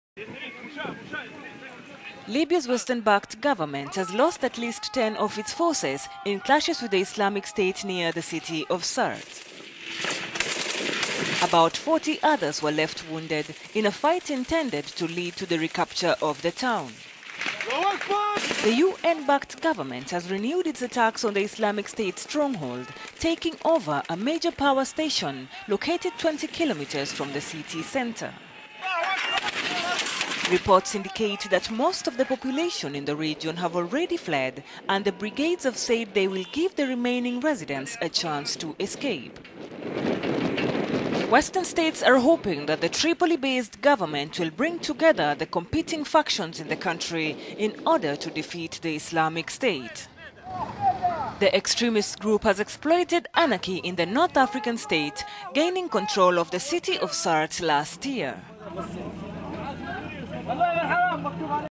Libyan forces lose 10 men in clashes with Islamic State near Sirte
Ten members of Libya's western-backed government forces have been killed in clashes with the Islamic State near the town of Sirte.

About 40 other members of the force were wounded, in a fight intended to lead to the recapture of the town from the Islamist group.

Libya|n forces lose 10 men in clashes with ISIS near Sirte Al Arabiya English (@AlArabiya_Eng) June 2, 2016

The UN backed government has in recent weeks launched …